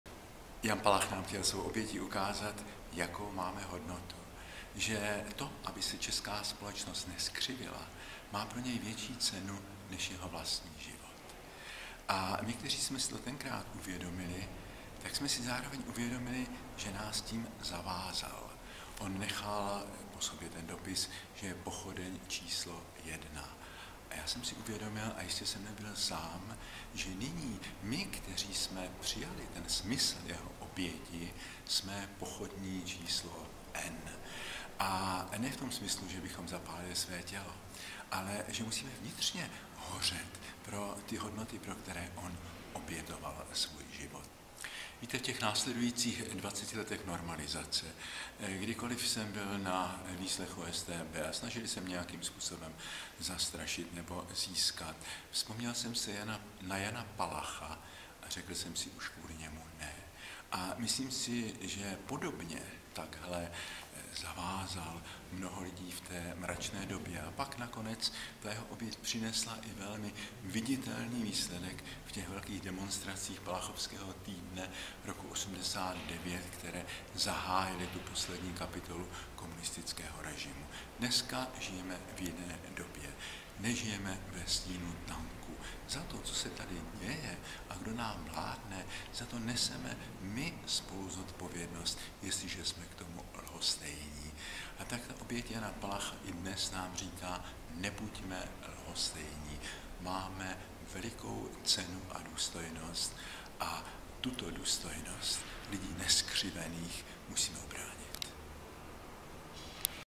Tomáš Halík o poselství Jana Palacha - 2019
Jan Palach nám chtěl svou obětí ukázat, jakou máme hodnotu. Že to, aby se české společnost nezkřivila, má pro něj větší cenu než vlastní život. (...) Jeho poselství dnes? Nebuďme lhostejní. Máme velikou cenu a důstojnost a tuto důstojnost lidí nezkřivených musíme ubránit."